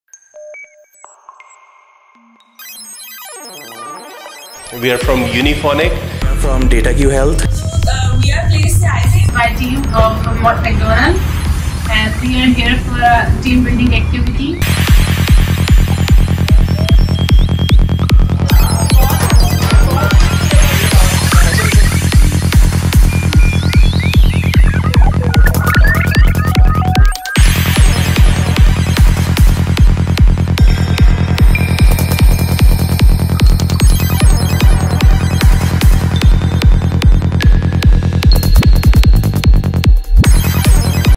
Corporate Team Building Activities at PanIQ Room
There is no better way to conduct Team Building activity other than Escape Room. Renowned companies like Unifonic, Data Health, We R Play and Mott Macdonald opted PanIQ Room for their staff team building events.